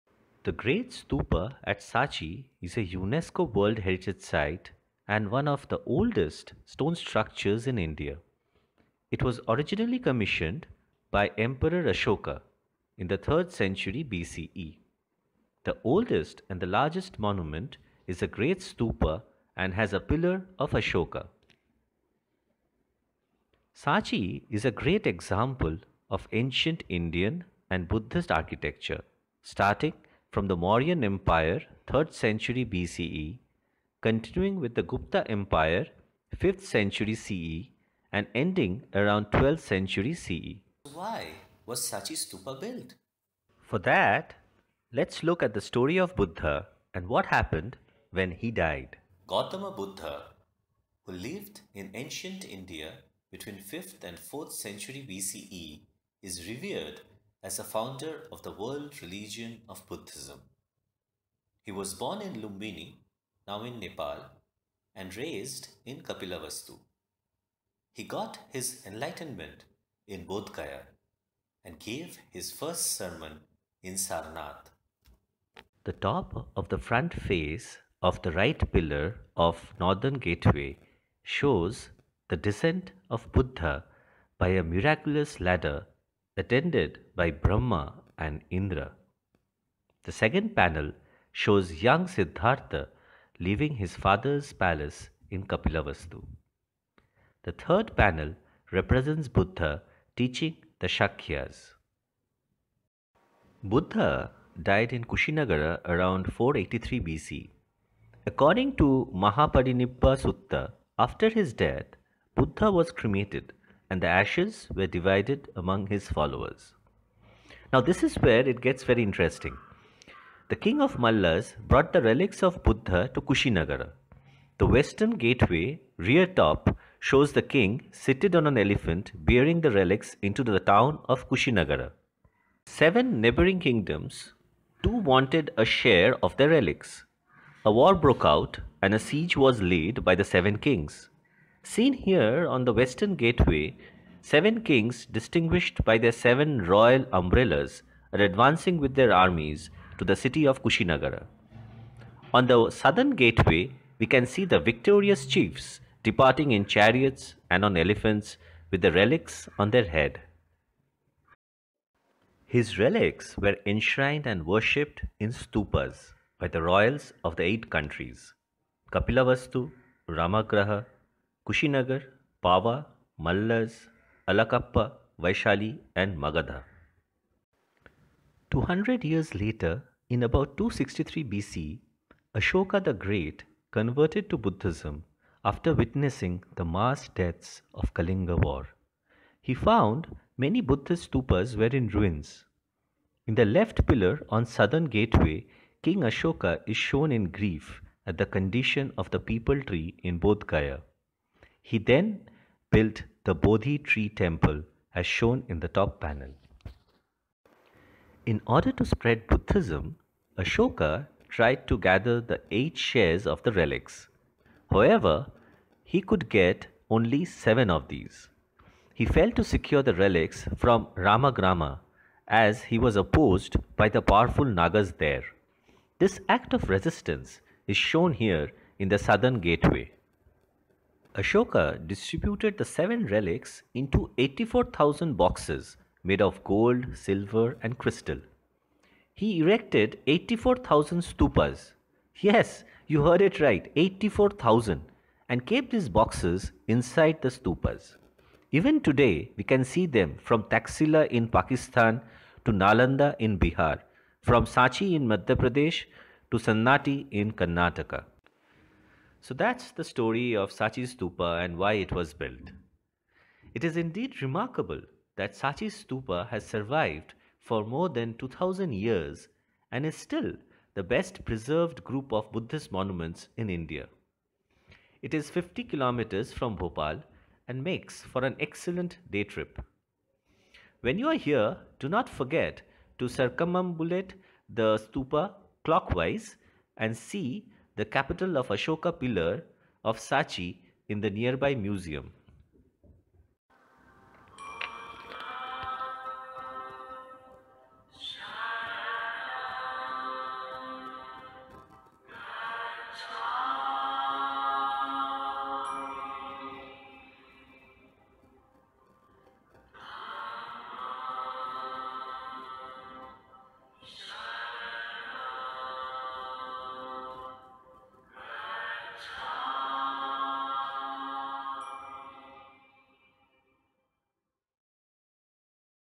Sanchi Stupa and why it was built | Story of Buddha | Ashoka & relics of Gautama Buddha
The Great Stupa at Sanchi is a UNESCO World Heritage Site and one of the oldest stone structures in India. It was originally commissioned by emperor Ashoka in the 3rd century BCE. The oldest, and the largest monument, is the Great Stupa and has a Pillar of Ashoka.

Sanchi is a great example of ancient Indian and Buddhist architecture starting from the Maurya Empire period (3rd century BCE), continuing with the Gupta Empire period (5th century CE), and ending around the 12th century CE.

So why was Sanchi Stupa built?

For that, let’s look at the story of Buddha and what happened when He died.
Gautama Buddha, who lived in ancient India between 5th and 4th century BCE, is revered as the founder of the world religion of Buddhism. He was born in Lumbini, now in Nepal, and raised in Kapilvastu. He got his enlightenment in Bodh Gaya and gave his first sermon in Sarnath.

The top of the front face of the right pillar of the Northern Gateway shows the descent of Buddha by a miraculous ladder attended by Brahma and Indra. The second panel shows young Siddhartha leaving his father’s palace in Kapilavastu. The third panel represents Buddha teaching the Shakyas.

Buddha died in Kusinagara around 483BC. According to Mahaparinibbana Sutta, after his death, Buddha was cremated and the ashes were divided among his followers.
Now this is where it gets very interesting!
King of the Mallas brought the relics of the Buddha to Kushinagara. The western gateway rear top shows the king, seated on an elephant, bearing the relics into the town of Kusinagara.
Seven neighbouring kingdoms wanted their share of the relics. A war broke out and a siege was laid by the seven kings. Seen here on Western Gateway, seven kings distinguished by their seven royal umbrellas, are advancing with their armies to the city of Kushinagara. On the Southern Gateway, we can see the victorious chiefs departing in chariots and on elephants, with the relics on their heads.
His relics were enshrined and worshipped in stupas by the royals of eight countries: Kapilavastu, Ramagrama, Kushinagar, Pava, Mallas, Allakappa, Vaishali and Magadha.

200 Years later in about 263 BCE, Ashoka the great, converted to Buddhism after witnessing the mass deaths of Kalinga War. He found many Buddhist stupas were in ruins. In the left pillar on Southern Gateway, King Ashoka is shown in grief at the condition of the pipal tree in Bodh Gaya. He then built the Bodhi tree temple as shown in the Top panel.

In order to spread the Buddhism, Ashoka tried to gather the eight shares of the relics. However, he could obtain only seven of these. He failed to secure the relics from Ramagrama as he was opposed by the powerful Nagas. This act of resistance is shown here in the Southern Gateway.

Ashoka distributed the seven relics into 84,000 boxes made of gold, silver and crystal. He erected 84,000 stupas, yes, you heard it right 84000 and kept these boxes inside the stupas. Even today, we can see them from Taxila in Pakistan to Nalanda in Bihar, from Sanchi in Madhya Pradesh to Sannati in Karnataka.
So that’s the story of Sanchi Stupa and why it was built.

It is indeed remarkable that Sanchi Stupa has survived for more than 2,000 years and is still the best preserved group of Buddhist monuments in India. It is 50 Kms from Bhopal and makes for an excellent day trip.
When you are here, do not forget to circumambulate the Stupa clockwise and see the capital of Ashoka pillar in the nearby museum.